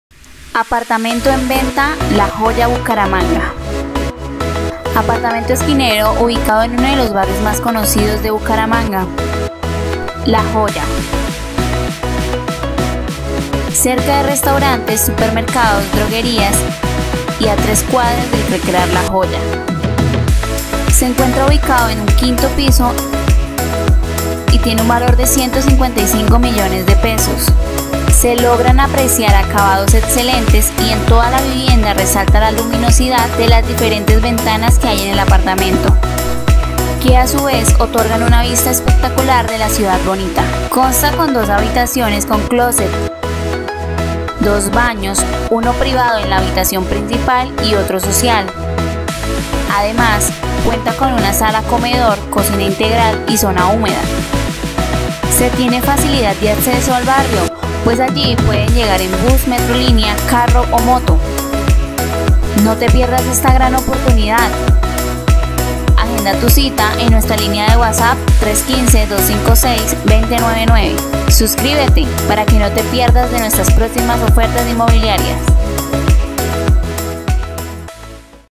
Apartamento en venta- La joya, Bucaramanga
Apartamento esquinero, ubicado en uno de los barrios más conocidos de Bucaramanga: LA JOYA. Cerca de restaurantes, supermercados, droguerías y a 3 cuadras del recrear la joya.

Se encuentra ubicado en un quinto piso y tiene un valor de 155´000.000. Se logran apreciar acabados excelentes y en toda la vivienda resalta la luminosidad de las diferentes ventanas que hay en el apartamento, que a su vez otorgan una vista espectacular de la ciudad Bonita.

Consta con 2 habitaciones con closet, 2 baños, uno privado en la habitación principal y otro social. Además, cuenta con sala-comedor, cocina integral y zona húmeda.

Se tiene facilidad de acceso al barrio, pues allí puedes llegar en bus, Metrolínea, carro o moto.